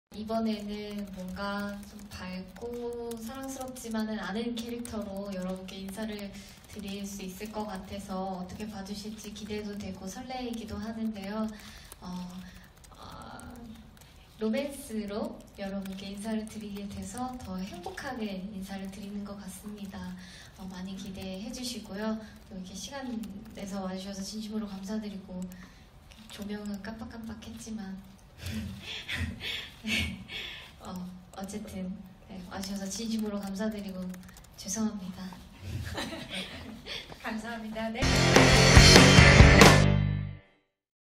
[아주스타 영상] 박보영, '러블리' 버렸다?! '너의 결혼식'으로 이미지 변신!
'러블리 대명사' 박보영, '너의 결혼식'으로 이미지 변신 나선다!